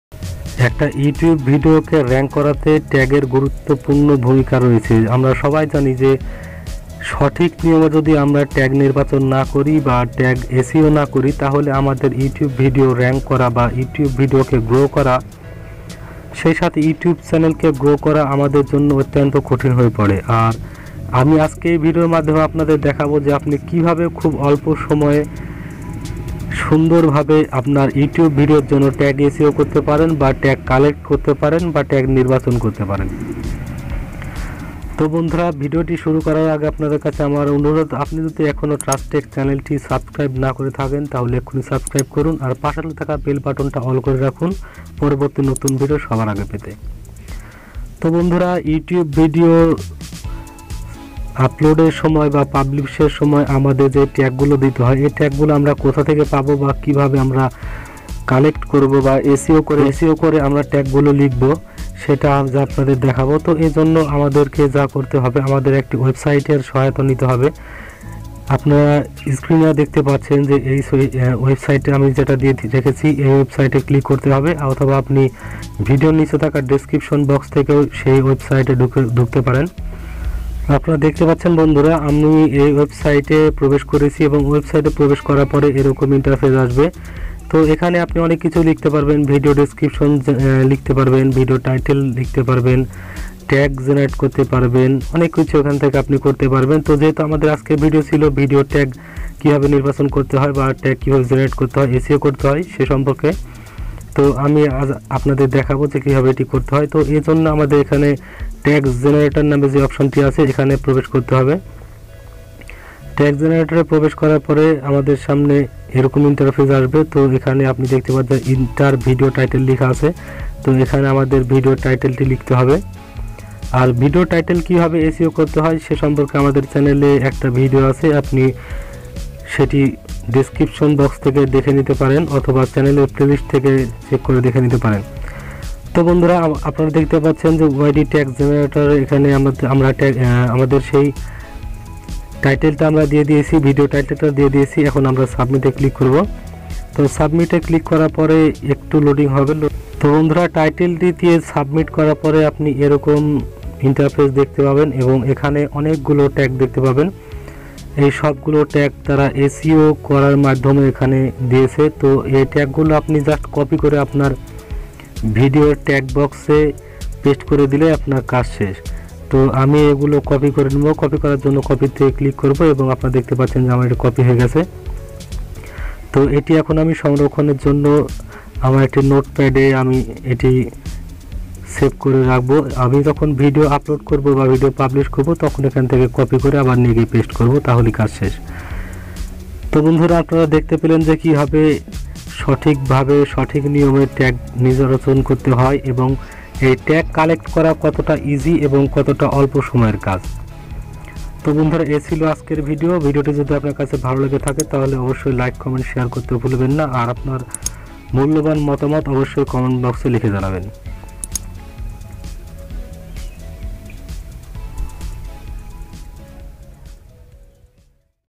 ভিডিওর Tag SEO করার নতুন সহজ উপায়। How To Tag SEO For Youtube Video.
SEO is a big challenge for everyone. So it is really important to use meta tag and keywords optimisation for your videos and webpages. This video will teach you how to use meta tags, keywords and other advanced techniques